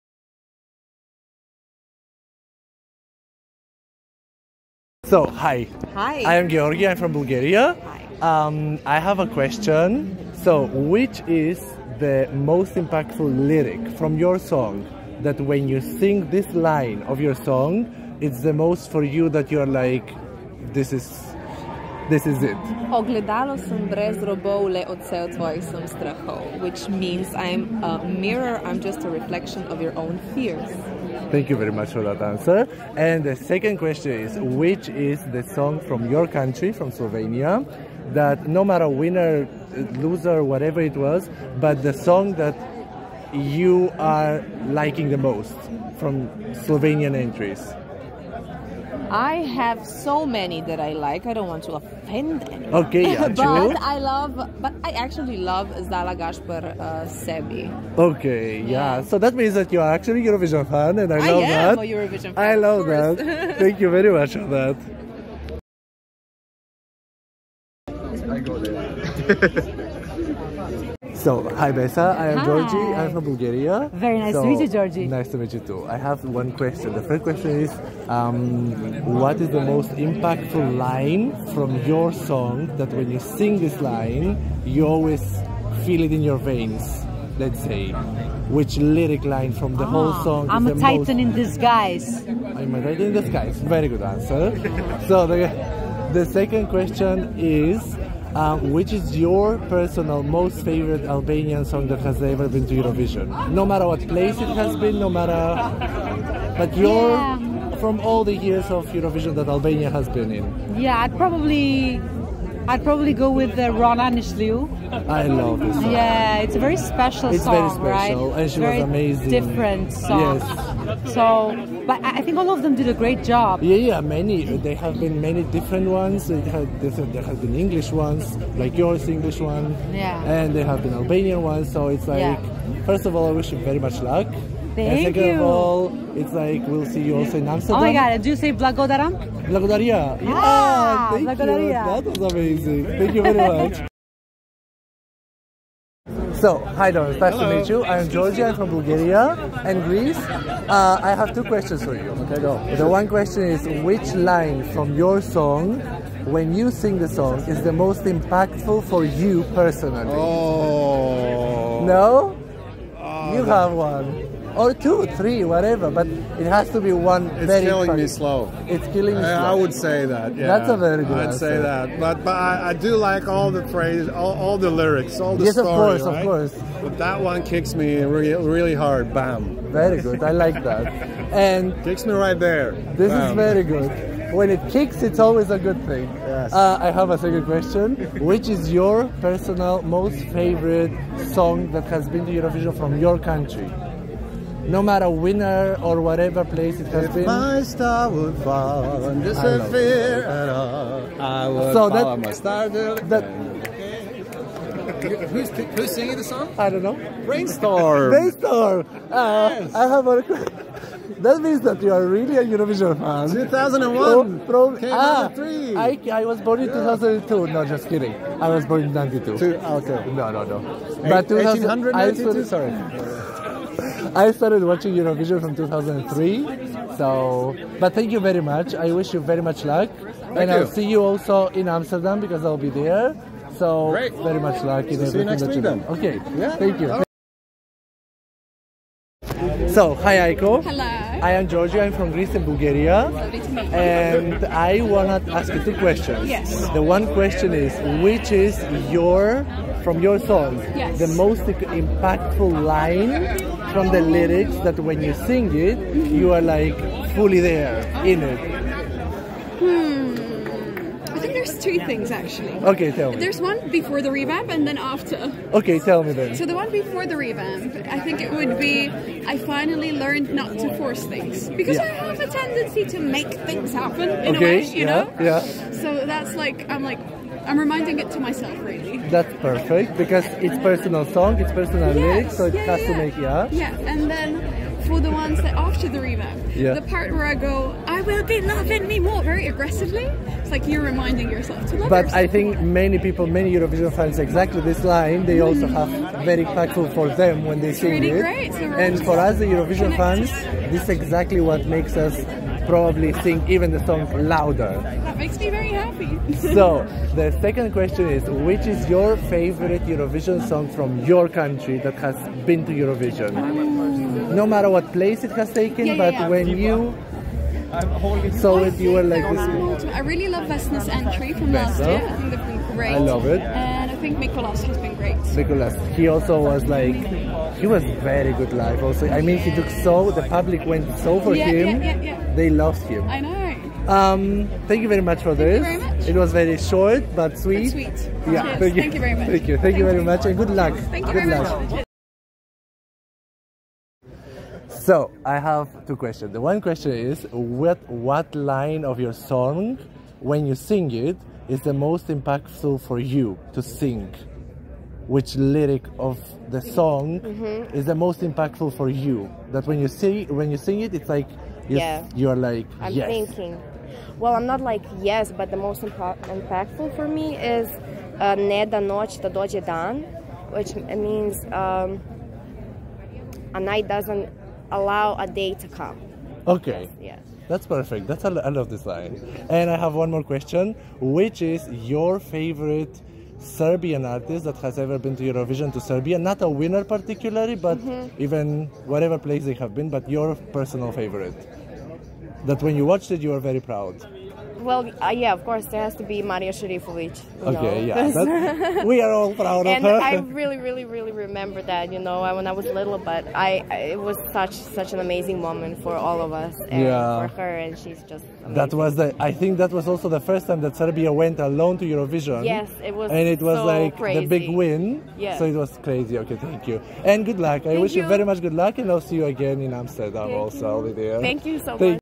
Barcelona EUROVISION Pre Party 2024 INTERVIEWS (Teya Dora-Dons-Raiven-Besa-Aiko)
When I had the time and the artist I had my 2 Questions that I wanted to ask and I did! So sweet of them to make the time for it and answer I hope you enjoy it.